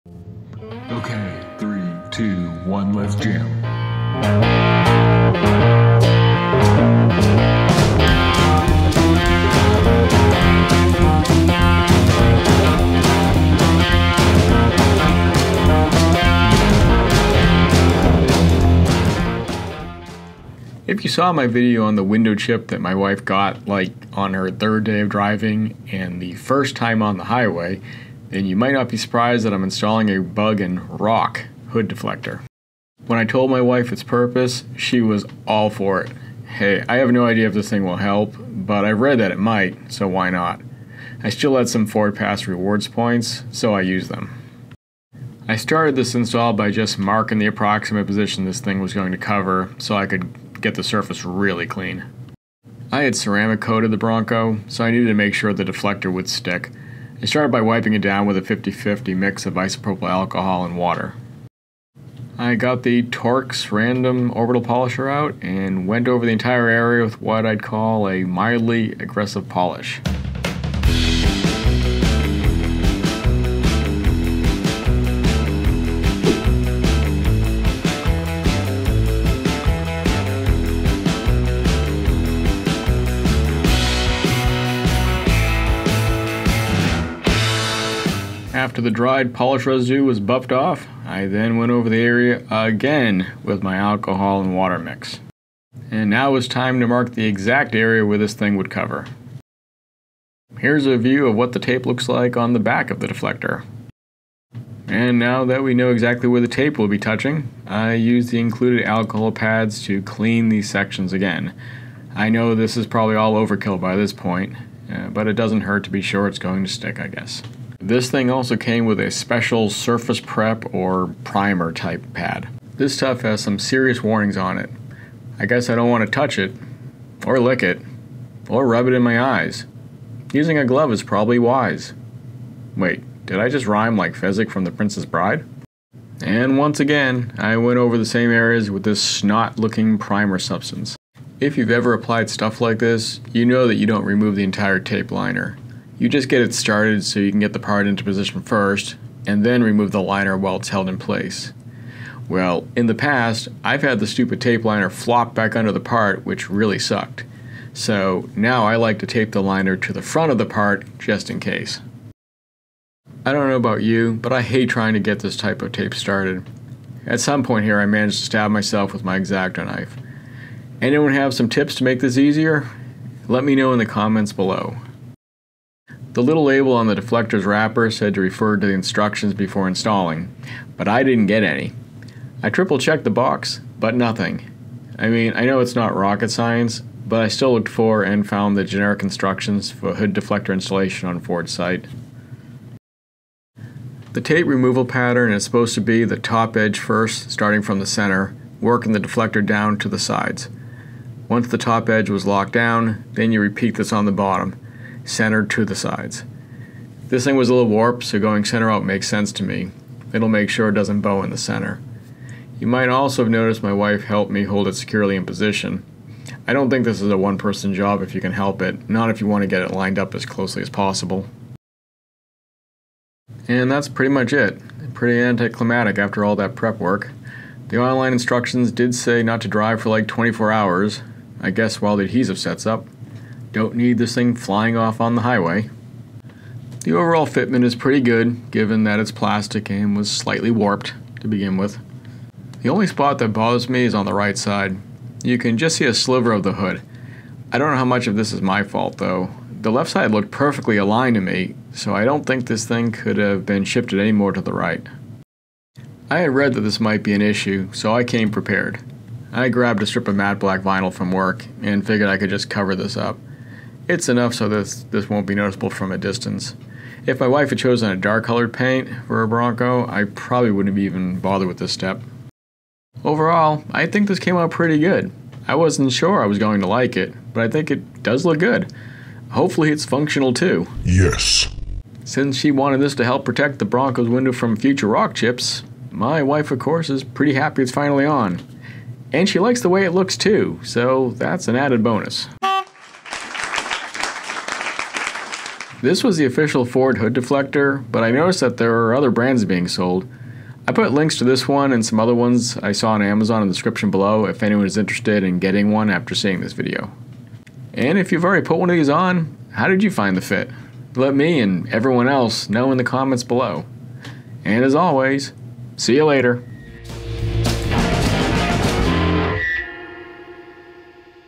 Ford Bronco Hood Armor Bug Rock Deflector Guard - Install and Review
In this video I give the Bronco some indirect windshield protection from flying bugs and rocks. Not only will this hopefully be a cool looking mod, but functional too!

*Product links if you'd like to do this yourself:
BRONCO Aeroskin II Black Textured Trail-Armor Hood Deflector